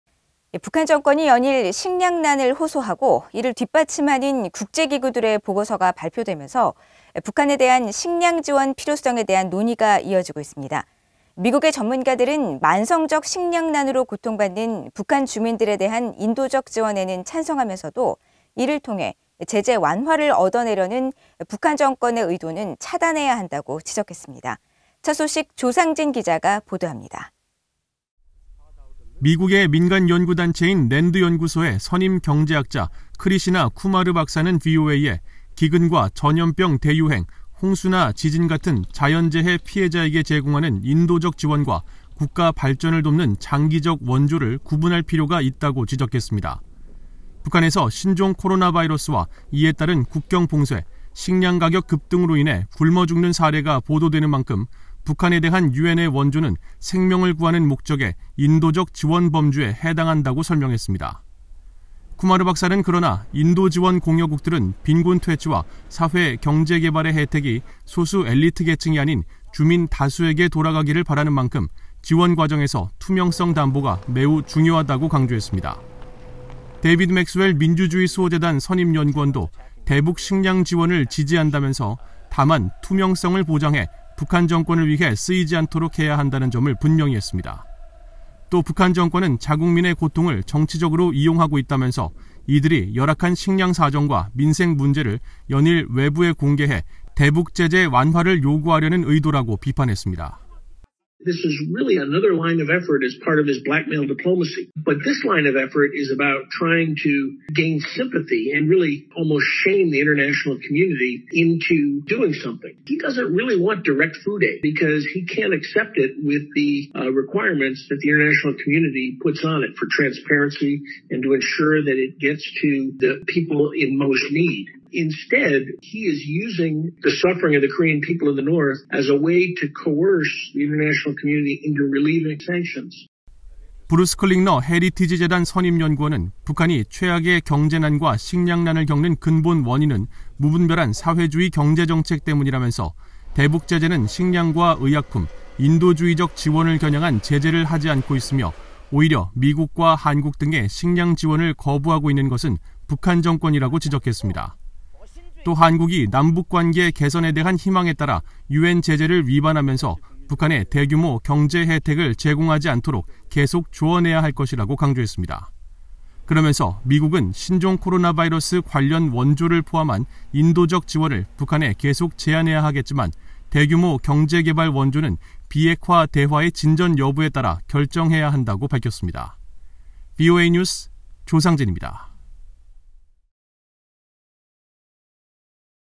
[VOA 뉴스] “대북 인도적 지원하되 ‘제재 완화 시도’ 차단”
북한 정권이 연일 식량난을 호소하고 이를 뒷받침하는 국제기구들의 보고서가 발표되면서 북한에 대한 식량 지원 필요성에 대한 논의가 이어지고 있습니다. 미국의 전문가들은 만성적 식량난으로 고통받는 북한 주민들에 대한 인도적 지원에는 찬성하면서도, 이를 통해 제재 완화를 얻어내려는 북한 정권의 의도는 차단해야 한다고 지적했습니다. 조상진 기자가 보도합니다. (영상편집: 김정규)